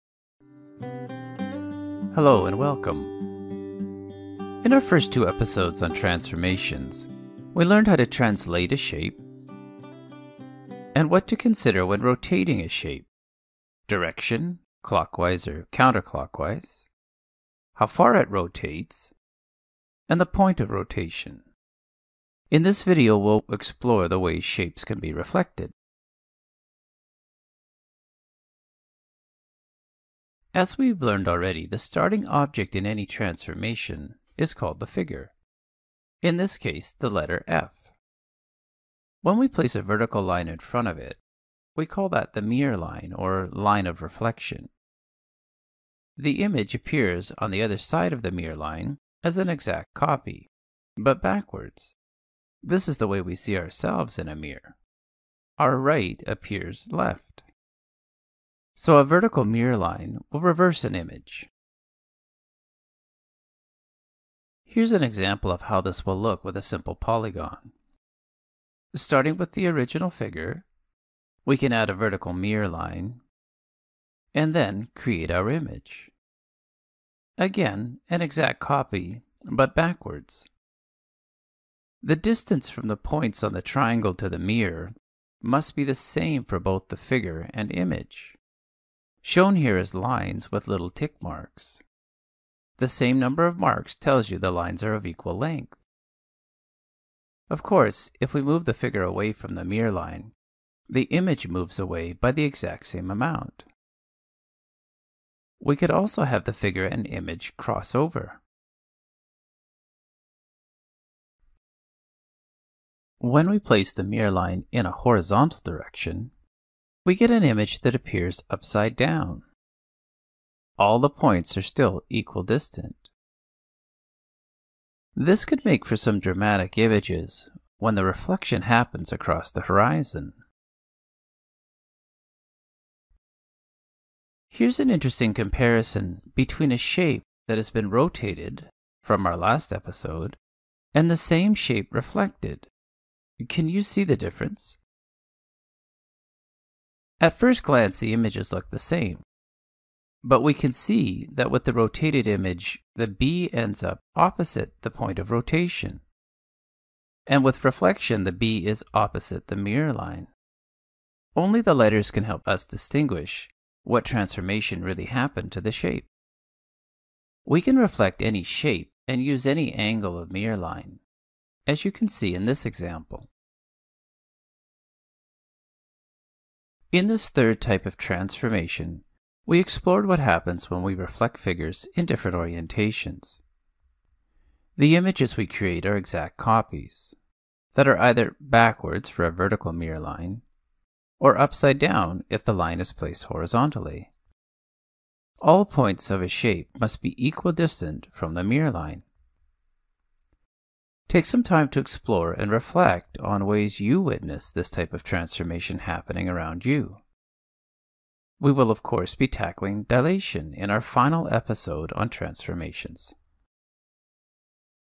WCLN - Geometry 6.3 - Reflection
Reflection is the third episode on Transformation in Geometry